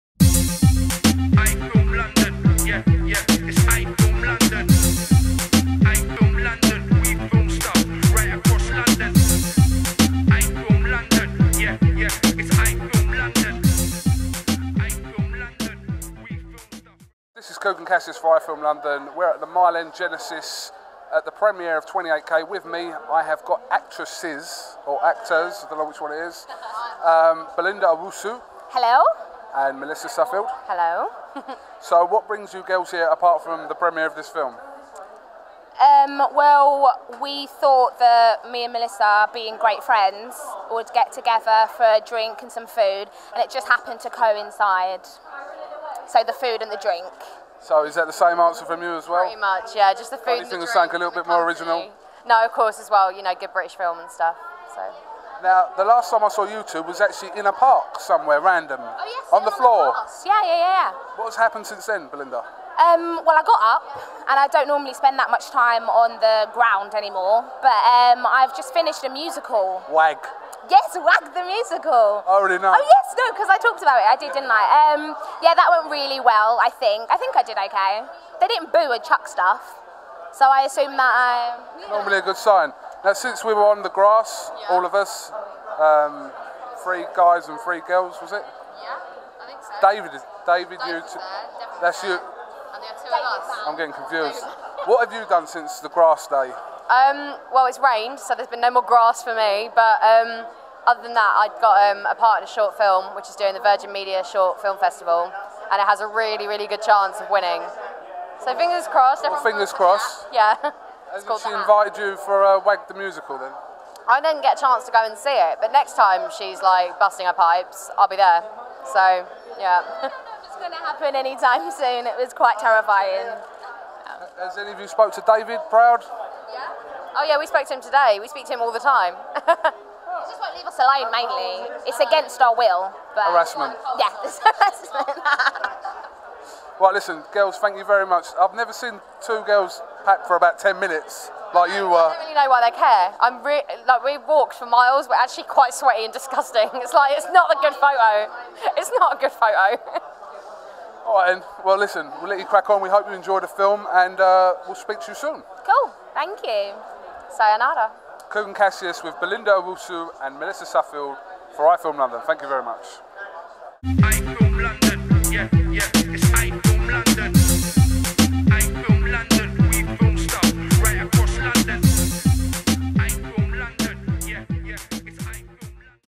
BELINDA OWUSU & MELISSA SUFFIELD INTERVIEW FOR iFILM LONDON / TWENTY8K PREMIERE
Actors Belinda Owusu and Melissa Suffield talk to Kugan Cassius and iFilm London at the official premiere of 'TWENTY8K' which was held at the Genesis Mile End, as part of the East End Film Festival, on July 5th 2012.